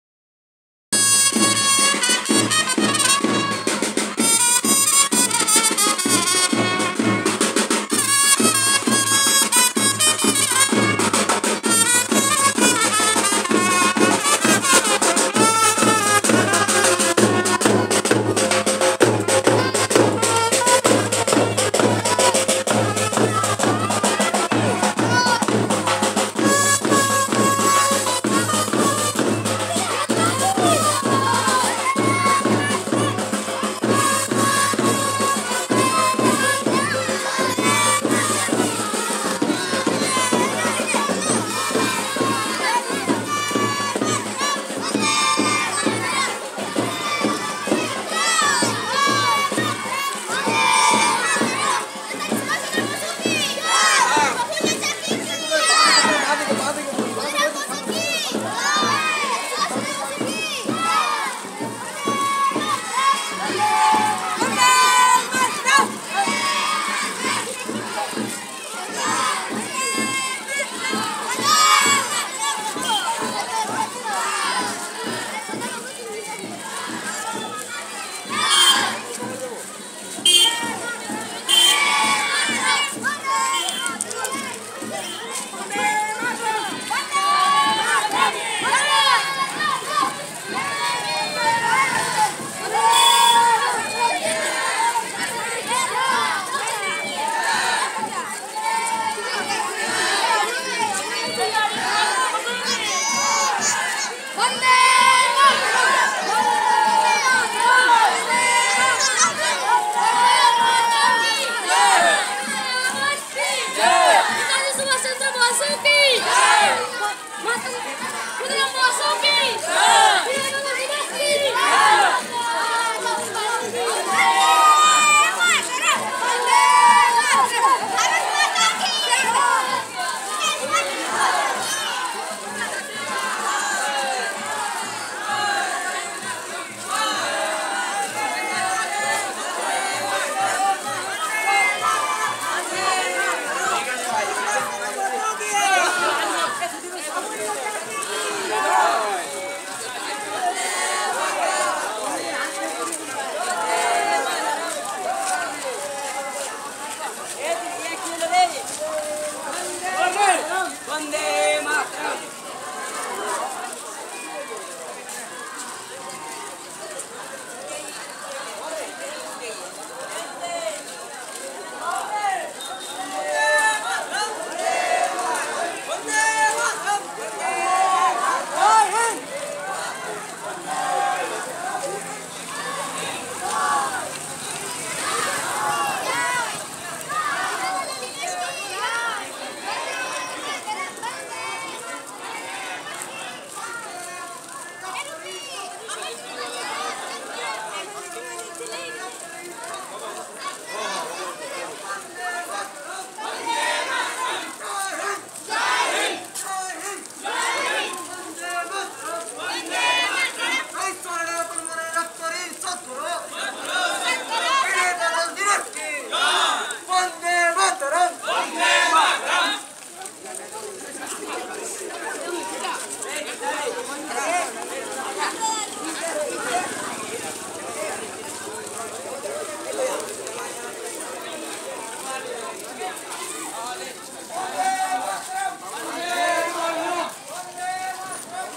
"5-Minute Independence Rally: Unveiling the Spirit of School Children's Patriotism"
Discover the essence of patriotism and unity in our compelling YouTube video, showcasing an inspiring Independence Rally orchestrated by school children, condensed into an engaging 5-minute spectacle.

Immerse yourself in the fervor as these young champions come together to celebrate the spirit of independence with enthusiasm and pride.

From stirring speeches to lively performances, this video captures the heart of the rally in a concise yet impactful presentation.

Witness the zeal, camaraderie, and dedication of these future leaders as they commemorate the nation's freedom struggle.

Join us in reliving the energy of this remarkable event and let it ignite your own patriotic spirit.